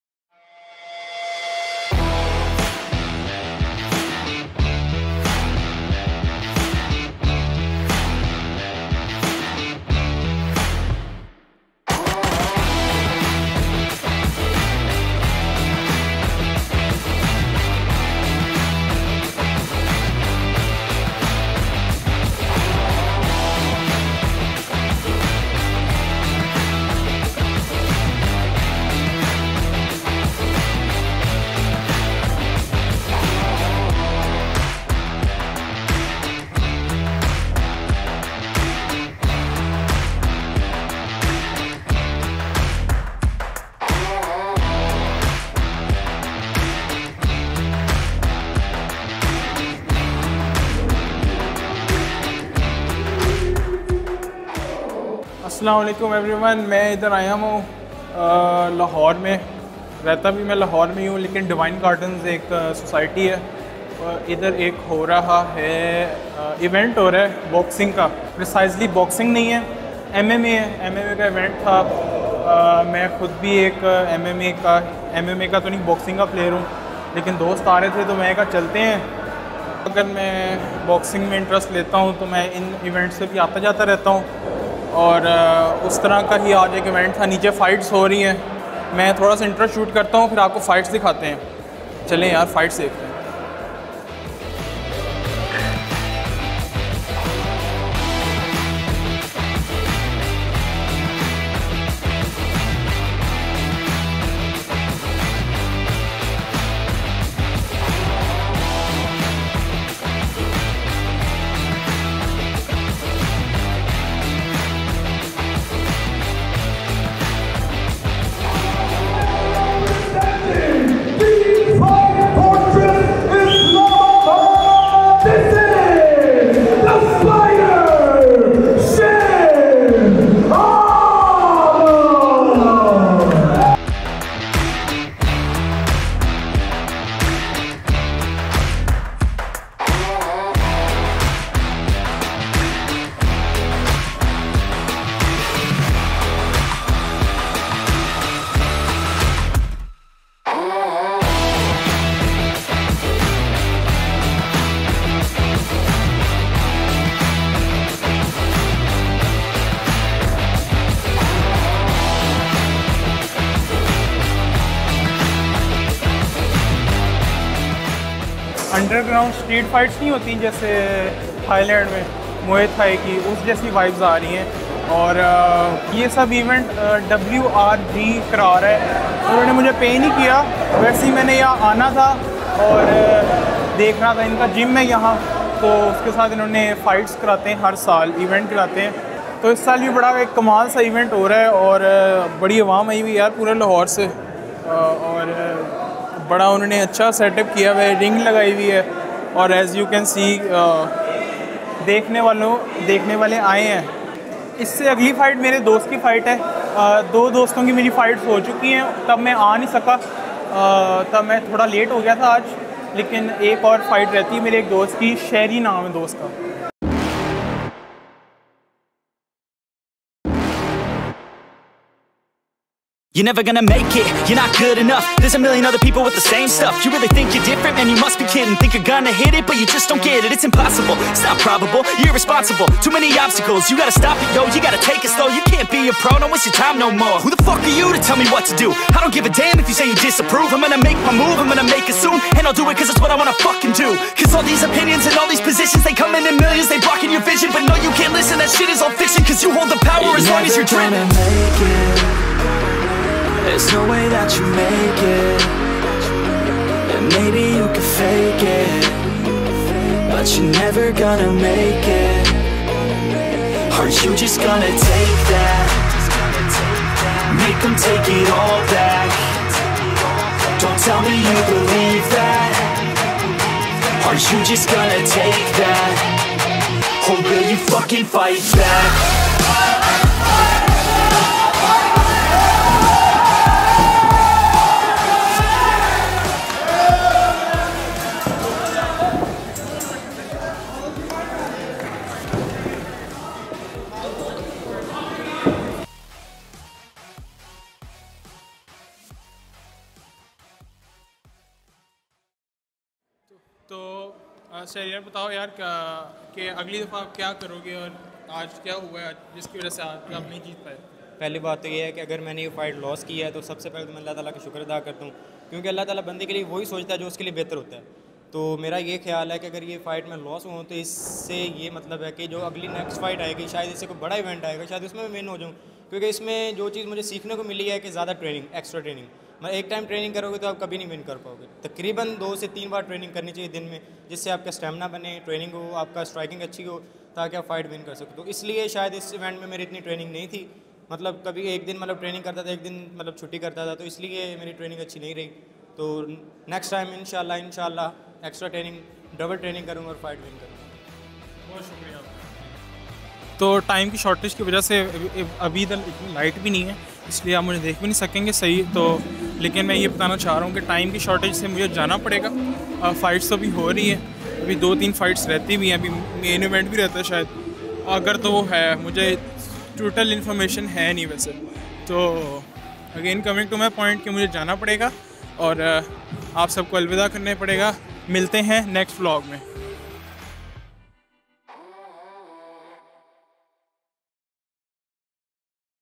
FINALLY!!! MARTIAL ARTS COMING BACK INTO PAKISTAN | WRG | Ahmed Subhan
About the Video :
So as you guys know that I am interested in martial arts. I have been in this sport for four years now, not doing it professionally or not for making a future in this sport but taking it as a hobby to keep myself fit and healthy. So that aside, a month ago there was an event consisting of Mixed Martial Arts (MMA) in Lahore . My friends (gymmates) were having fights in that event . So, as a means to support them, I too went on with them. But this time, I took my Vlogging Equipment with me so that I can Vlog the event. Above is the result . . Enjoy and don't forget to tell me your views in the comments.